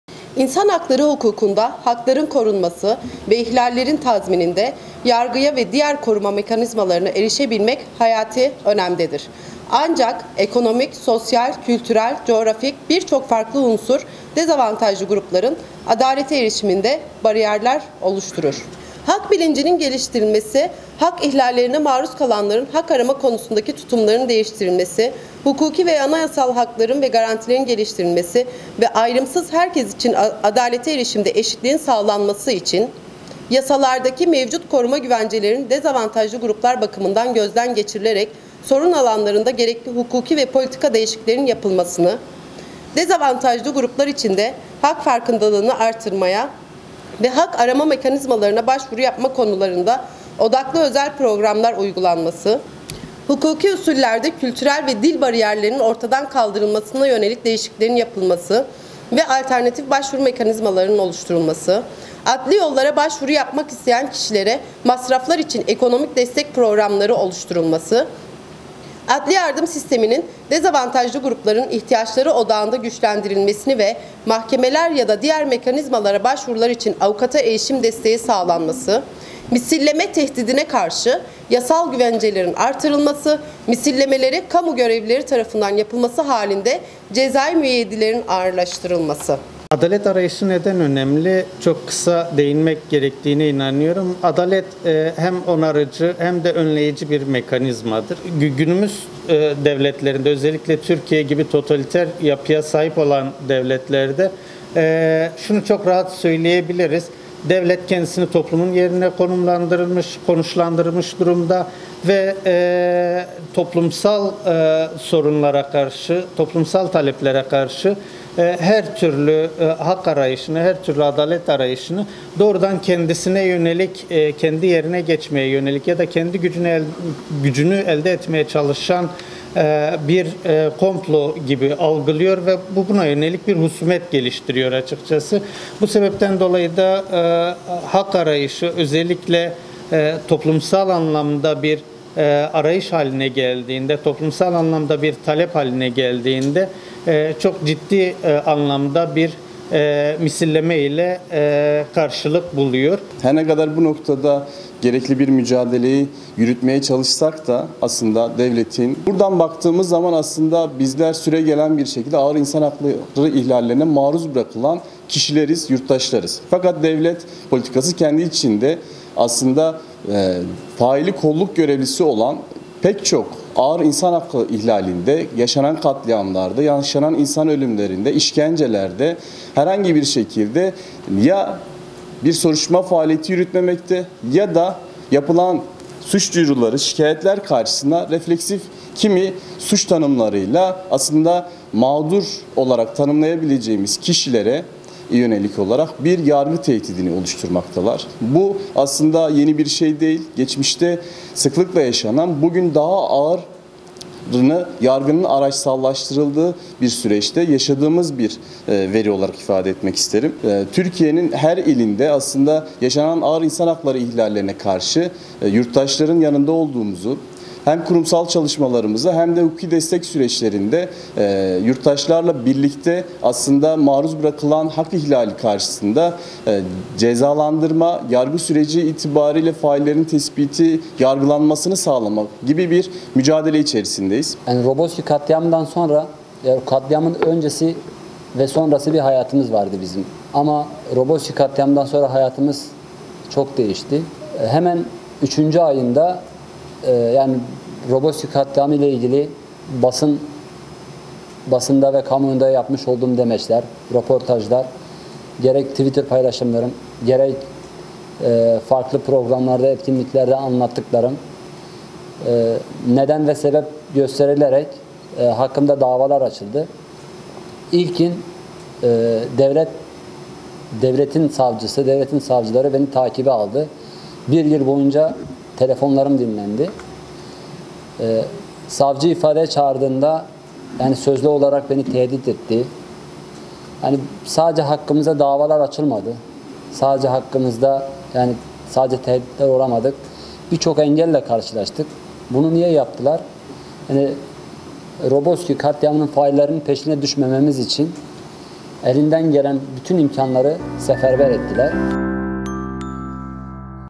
AMED'DE 'HAK' PANELİ: ARAYIŞTAN VAZGEÇİLMEMELİ
Amed'de "Hak Arama Özgürlüğünün Güçlendirilmesi" kampanyası kapsamında yapılan panelde konuşan Veli Encü, "Hak arayışımızdan asla vazgeçmememiz gerekiyor" dedi.